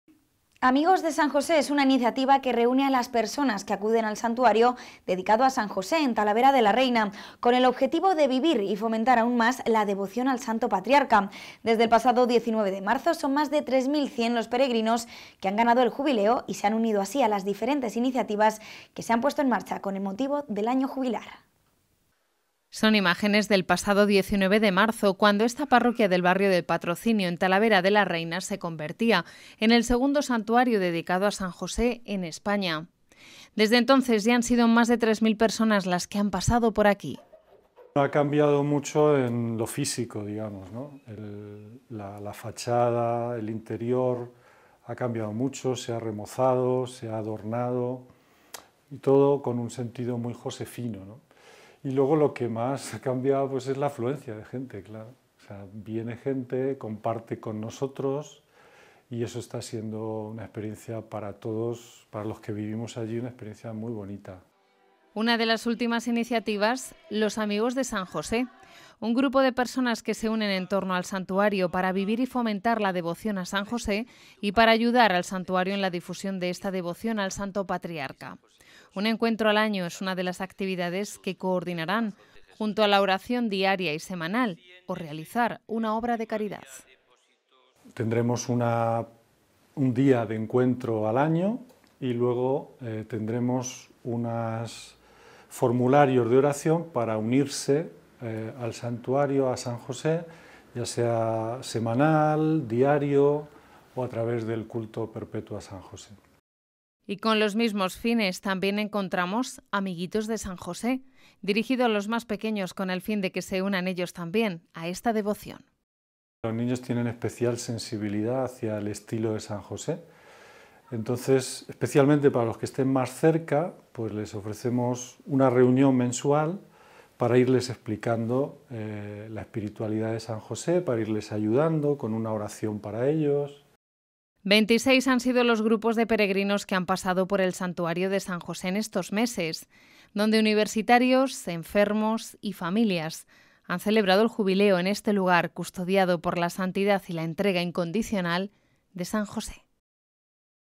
Nace la Asociación de Amigos de San José en torno al Santuario Diocesano
Fomentar la devoción a San José. Es el principal objetivo de la Asociación Los Amigos de San José que ha surgido en torno al Santuario Diocesano de Talavera de la Reina. Desde el pasado 19 de marzo, segundo santuario dedicado al santo custodio en nuestro país y templo jubilar, al que ya han acudido más de 3.100 peregrinos… Entre sus actividades, un encuentro al año de todos los que formen parte de la asociación que también ofrece la posibilidad de unirse a esta devoción a los más pequeños con actividades propias para ellos.